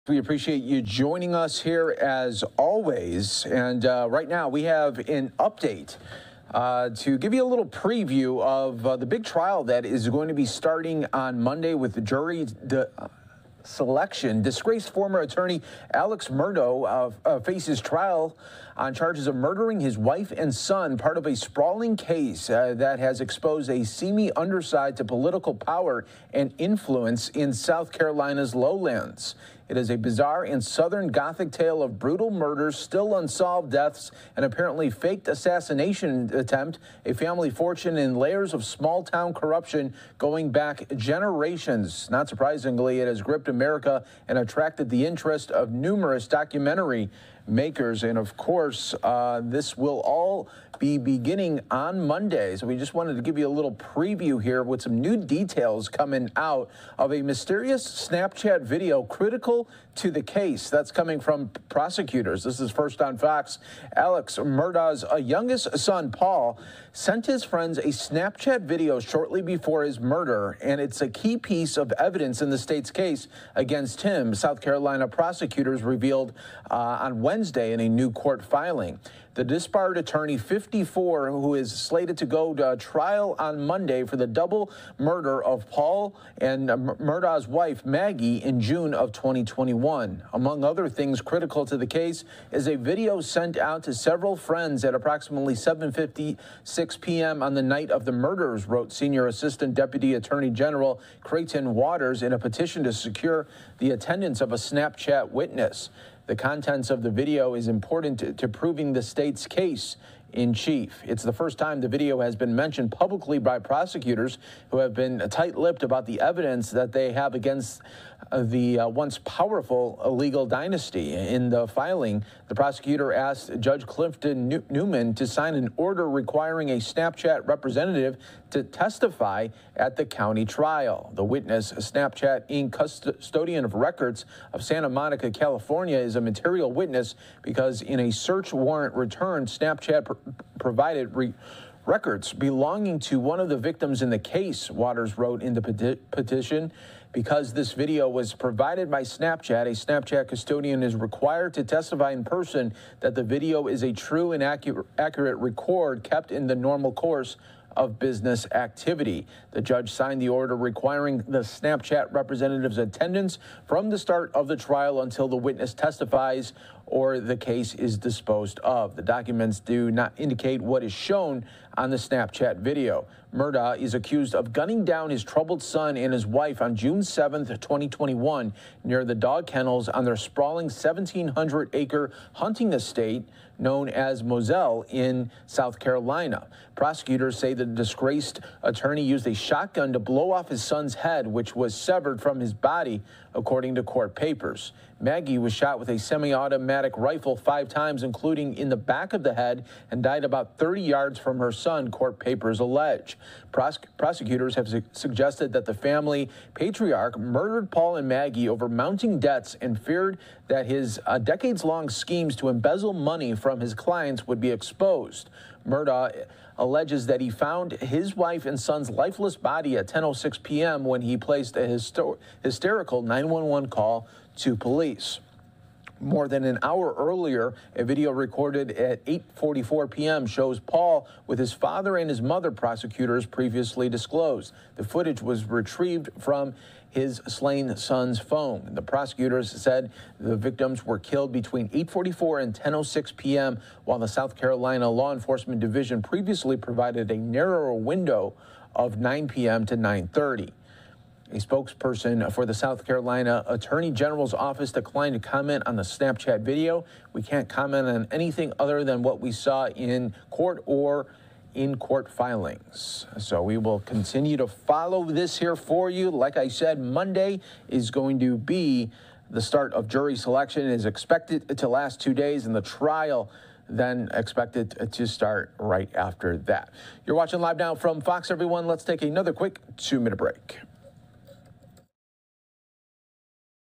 Murdaugh murders trial preview: Snapchat evidence hours before murders revealed | LiveNOW from FOX
Alex Murdaugh's youngest son Paul Murdaugh sent his friends a Snapchat video shortly before his murder — and it is a key piece of evidence in the state's case against him, South Carolina prosecutors revealed Wednesday in a new court filing.

The disbarred attorney, 54, is slated to go to trial Monday for the double slaying of his 22-year-old son and his wife, Maggie Murdaugh , 52, in June 2021.